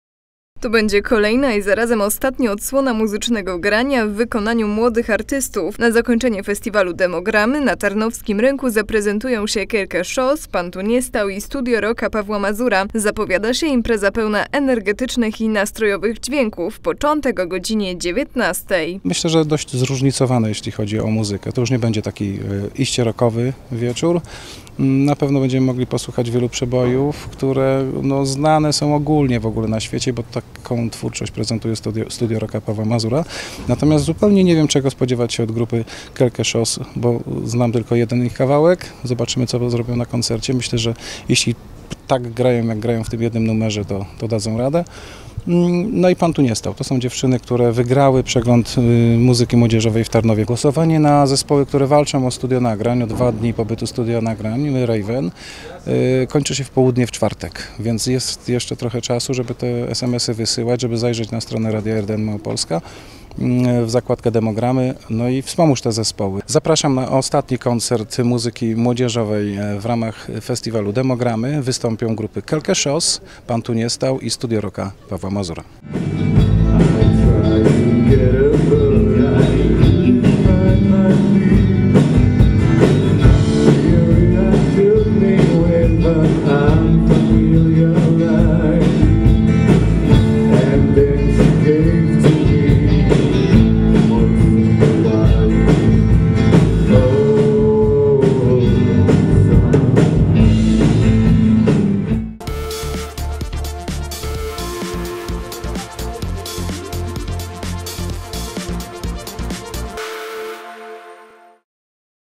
DeMoGramy na tarnowskim rynku - Magazyn Miejski 14/07/2015 - imav.tv
To będzie kolejna i zarazem ostatnia odsłona muzycznego grania w wykonaniu młodych artystów. Na zakończenie  Festiwalu DeMoGramy  na tarnowskim rynku zaprezentują się Quelque Chose, Pan Tu Nie Stał i Studio Rocka Pawła Mazura. Zapowiada się impreza pełna  energetycznych i nastrojowych dźwięków. Początek o godz. 19.00.